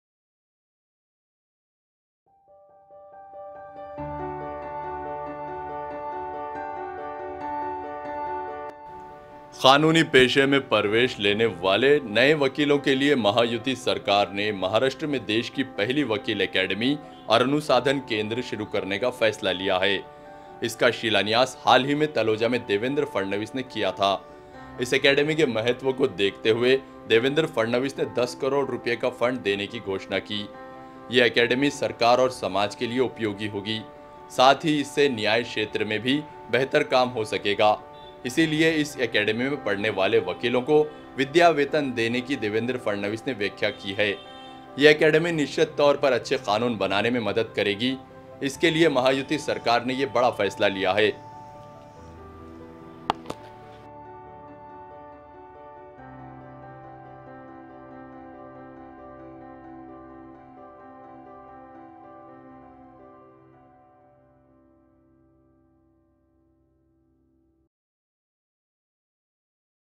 DevaBhau@DF_ वकीलों को मिलेगी शैक्षणिक पगार, देश की पहली अधिवक्ता अकादमी, देवेंद्र फडणवीस की पहल
PLZ SUBSCRBE OUR CHANNEL